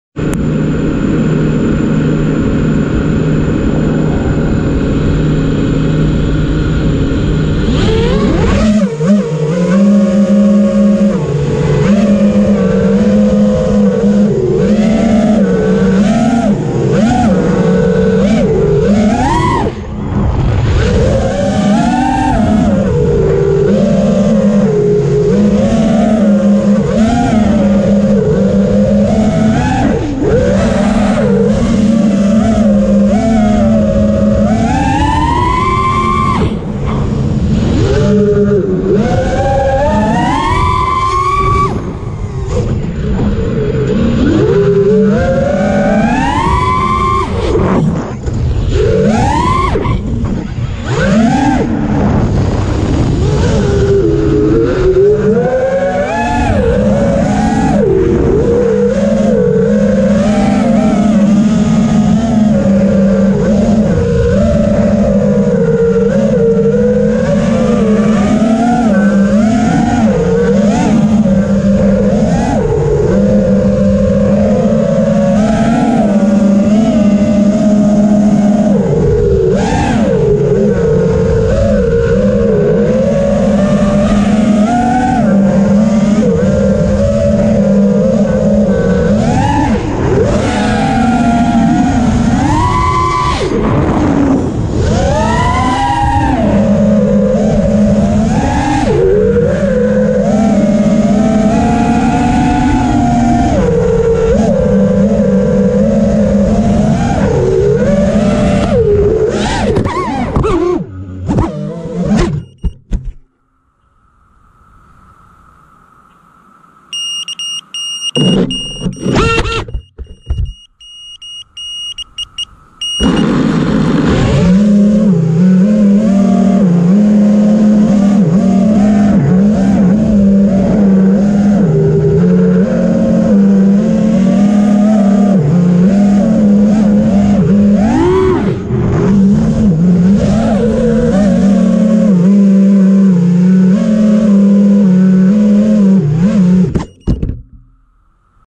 Turtle mode!!!!
Just some crusin . . . workin on flying smooth

Strix screech freestyle frame
Diatone mamba 405 stack on 6s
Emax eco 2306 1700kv
Johnny fpv props